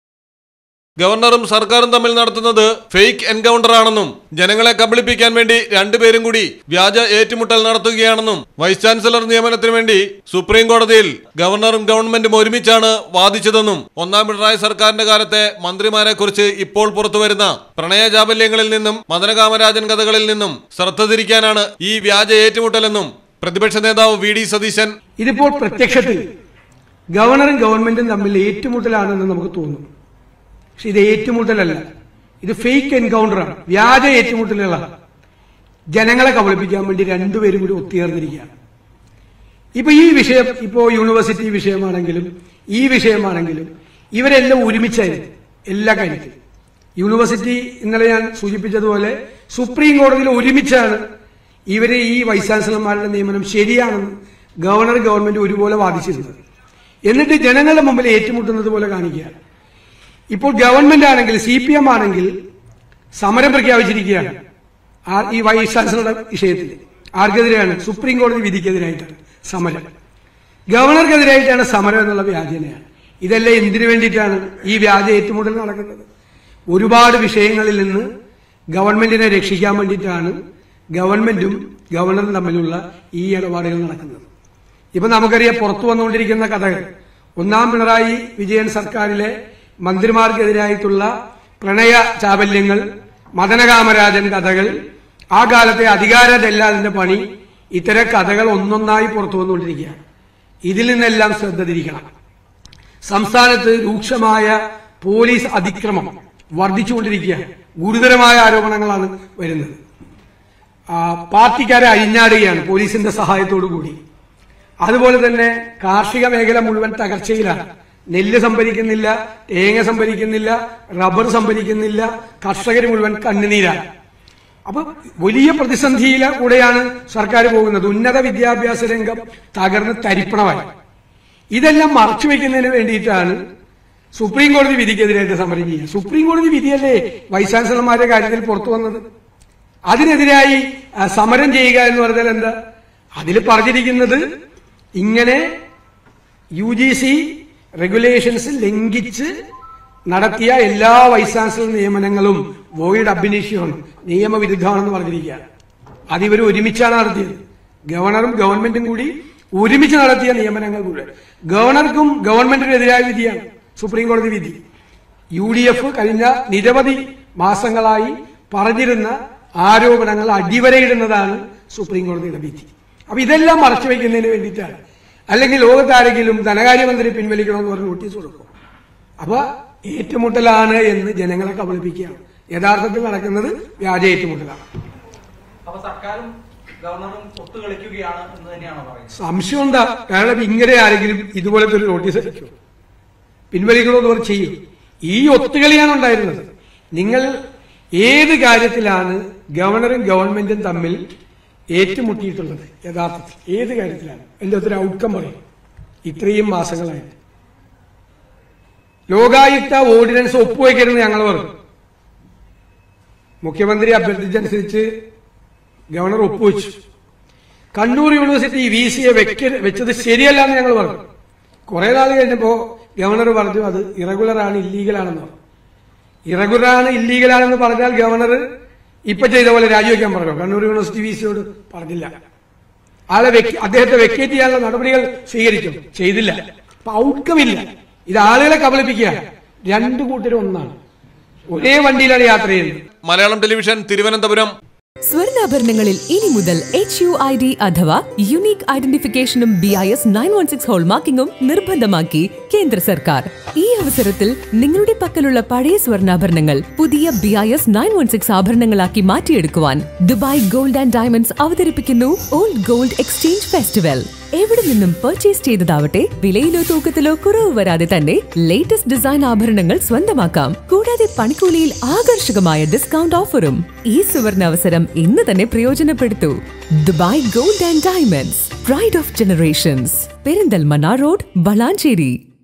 മുൻ മന്ത്രിമാരുടെ മദനകാമരാജൻ കഥകളിൽ നിന്ന് ശ്രദ്ധ തിരിക്കാനാണ് ഈ വ്യാജ ഏറ്റുമുട്ടൽ :: VD സതീശൻ.
മുൻ മന്ത്രിമാരുടെ മദനകാമരാജൻ കഥകൾ പുറത്ത് വരുന്നത്തിൽ നിന്ന് ശ്രദ്ധ തിരിക്കാനാണ് ഗവർണറും സർക്കാരും തമ്മിലുള്ള ഈ വ്യാജ ഏറ്റുമുട്ടൽ : VD സതീശൻ.